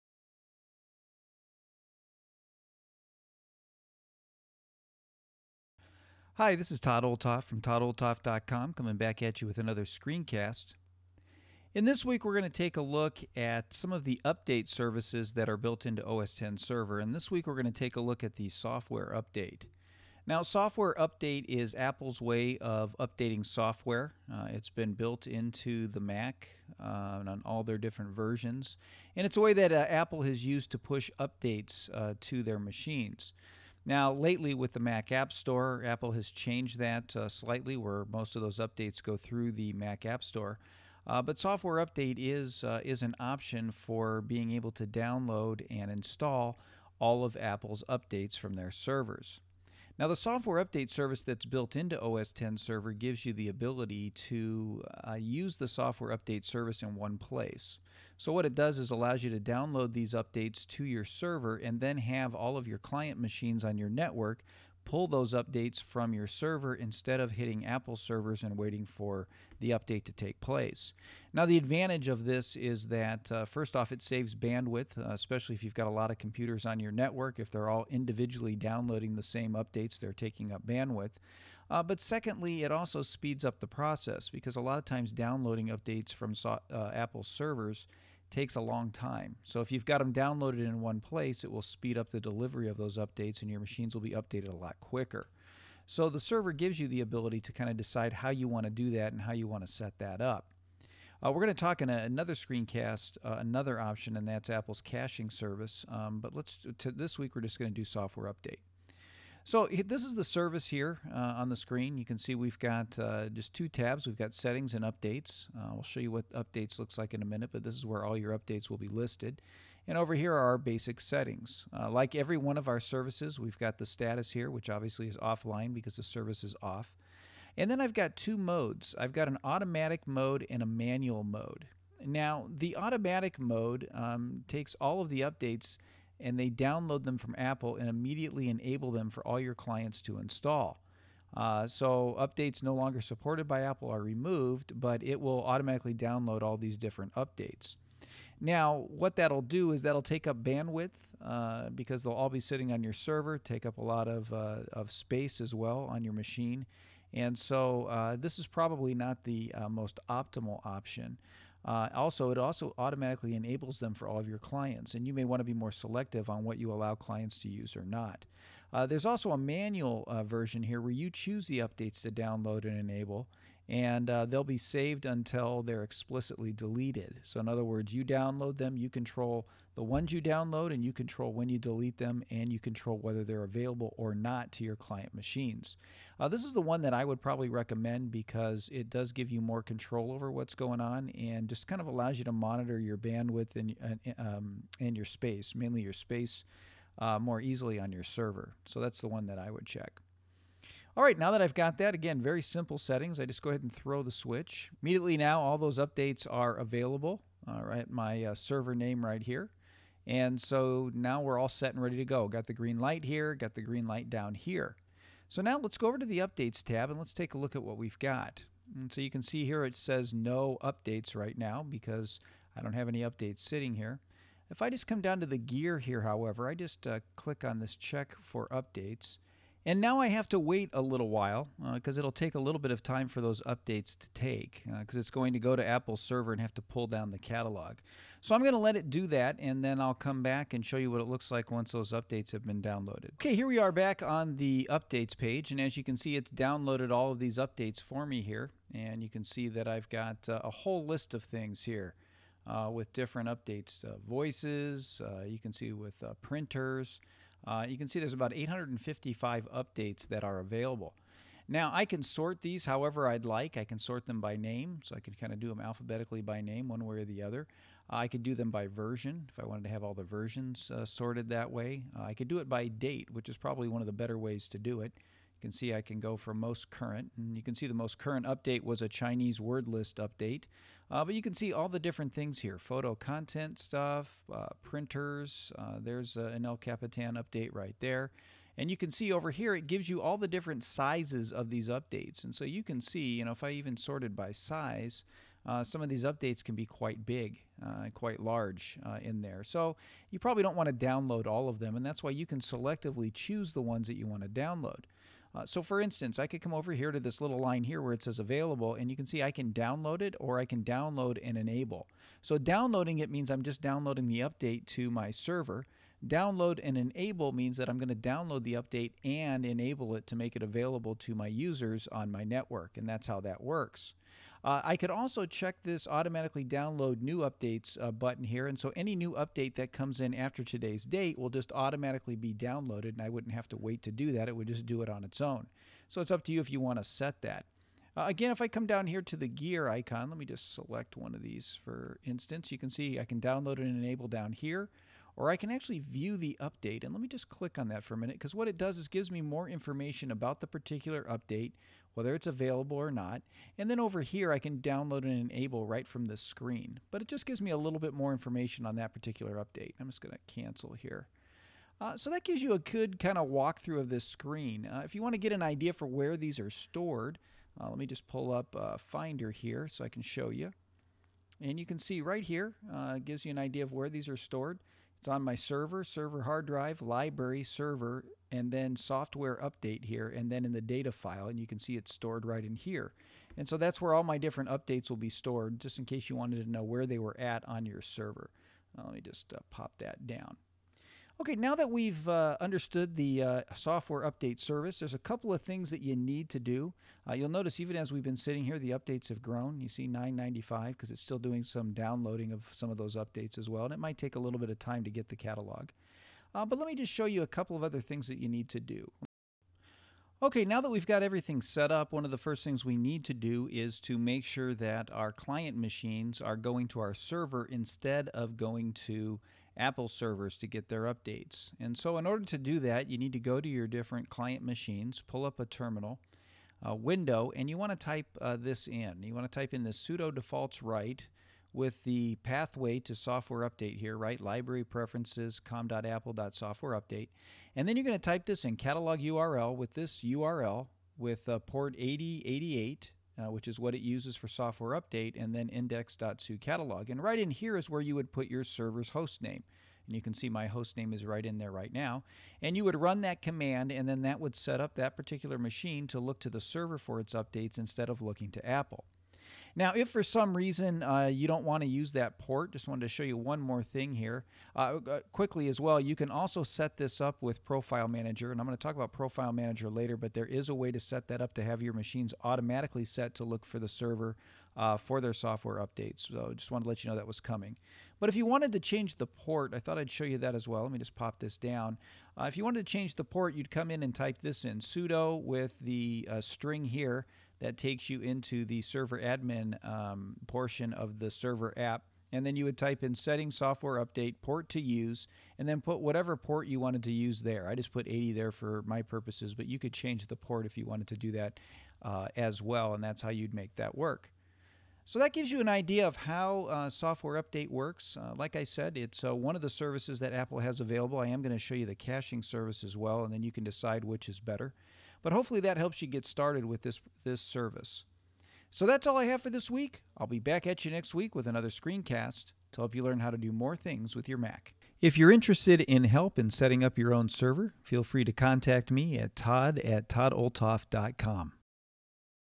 El Capitan Server Part 19: Software Update
In this screencast tutorial, I cover how to set up the built in Software Update Service. Software Update has been Apple's way of issuing updates to Macs, especially prior to the Mac App Store. Built into El Capitan Server is a Software Update Service that allows you to cache updates so your users download them from your server instead of Apple which limits the bandwidth needed for each user to download their own update. In this screencast I show you how to set up the service and some tips on how to customize it to fit your environment.

If you find this tutorial helpful please like, favorite and share it so others can find it on the internet.